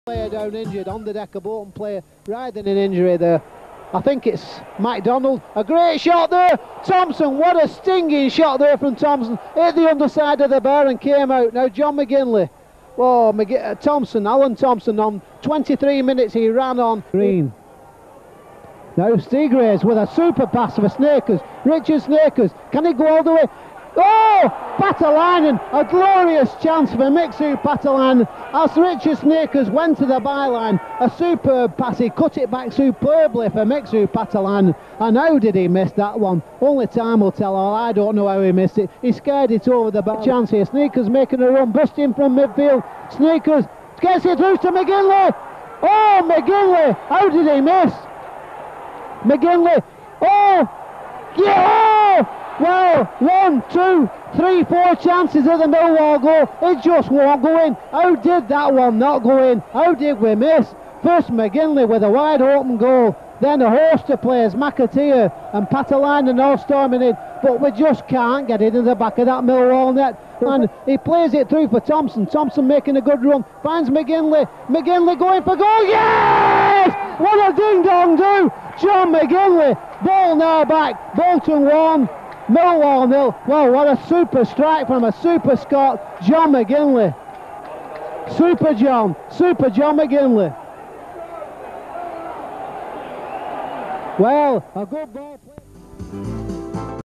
Millwall 0-1 Bolton Wanderers 1994/1995
19th March 1995 - Football League Division One
Millwall 0-1 Bolton Wanderers
Goal: John McGinlay
Attendance: 6,103
Commentator: Dave Higson